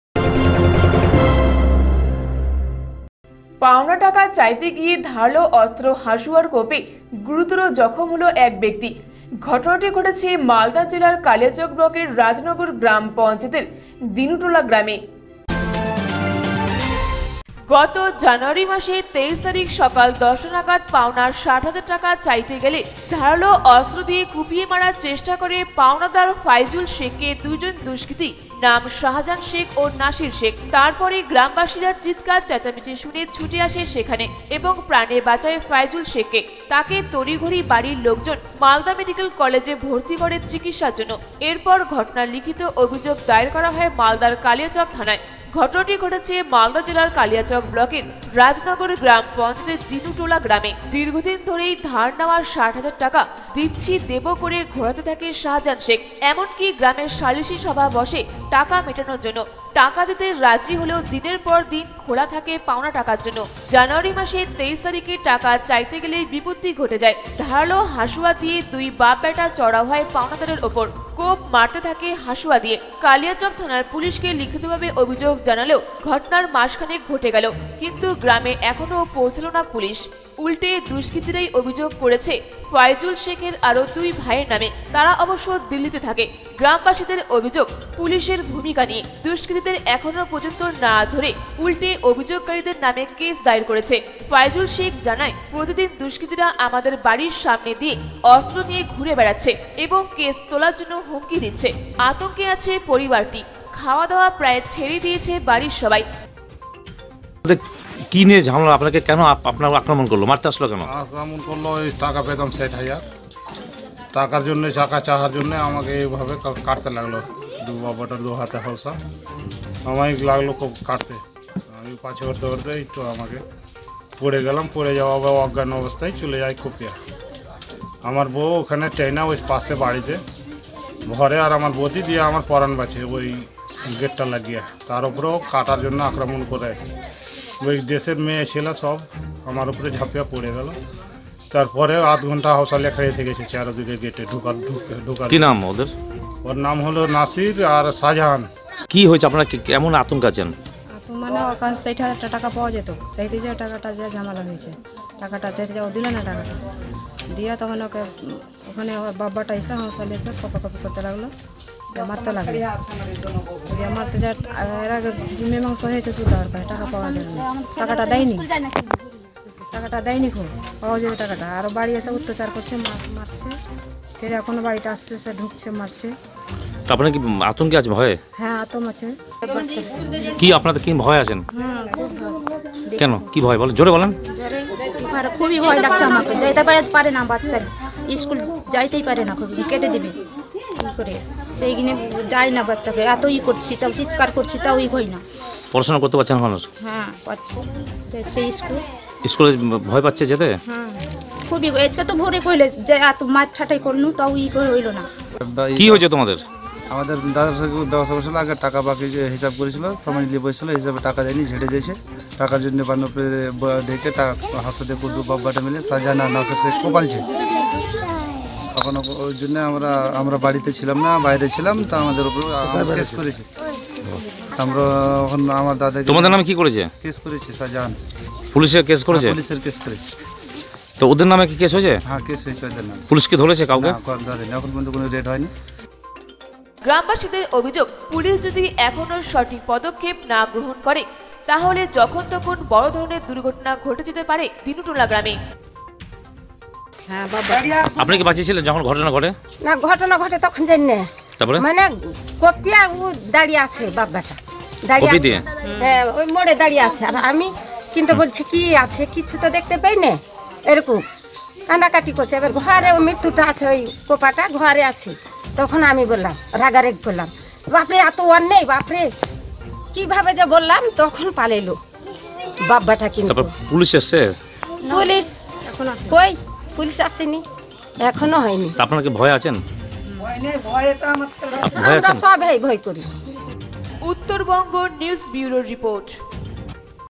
পাওনা টাকা চাইতে গিয়ে ধারালো অস্ত্র হাসুয়ার কোপে গুরুতর জখম এক ব্যক্তি
পাওনা টাকা চাইতে গিয়ে ধারালো অস্ত্র হাসুয়ার কোপে গুরুতর জখম এক ব্যক্তি ঘটনা মালদা জেলার কালিয়াচক ব্লকের রাজনগর গ্রাম পঞ্চায়েতের দিনু টোলা গ্রামের